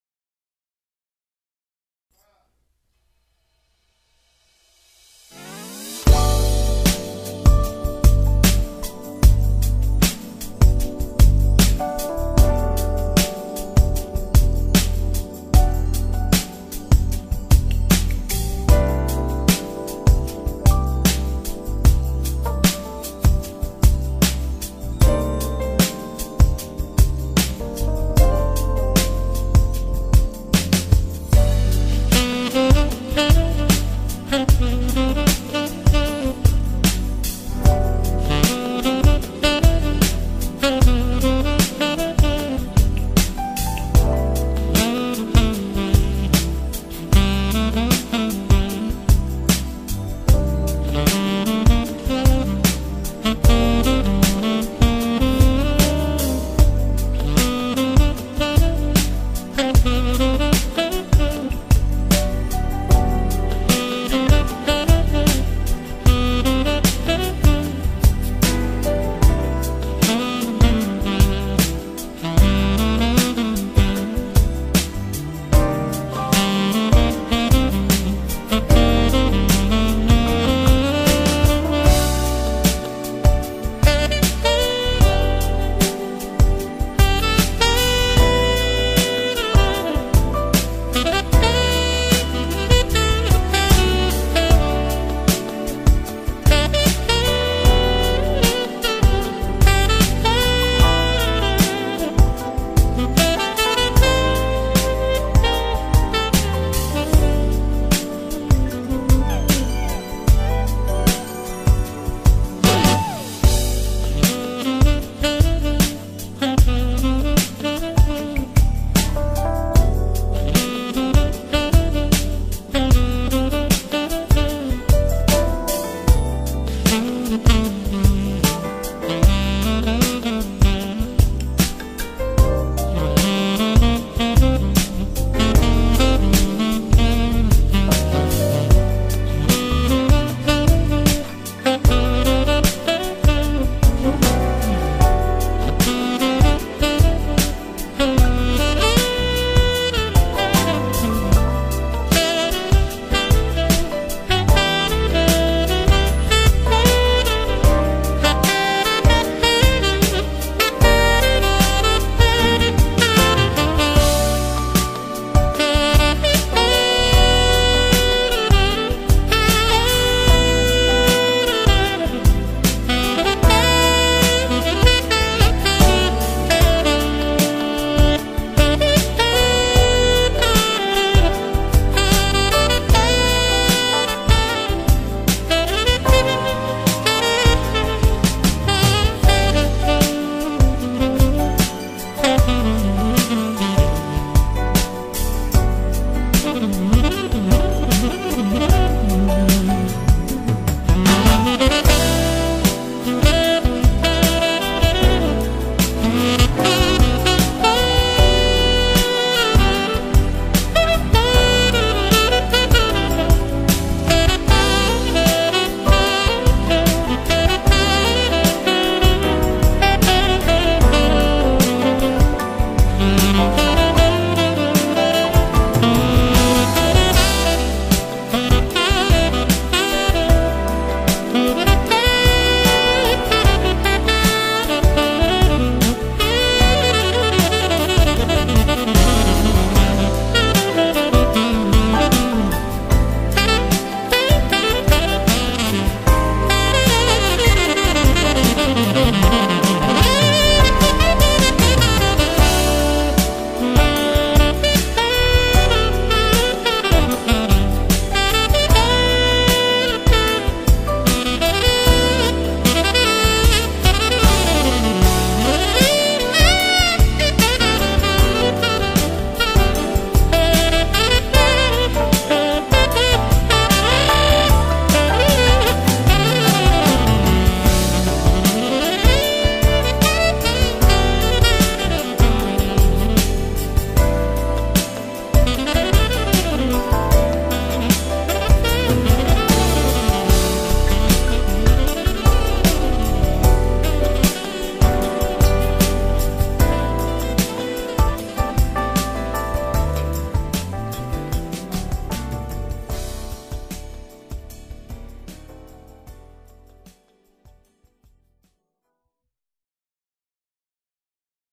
AMERICAN SMOOTH JAZZ
Here's some excellent smooth jazz by American smooth jazz saxophonist Euge Groove.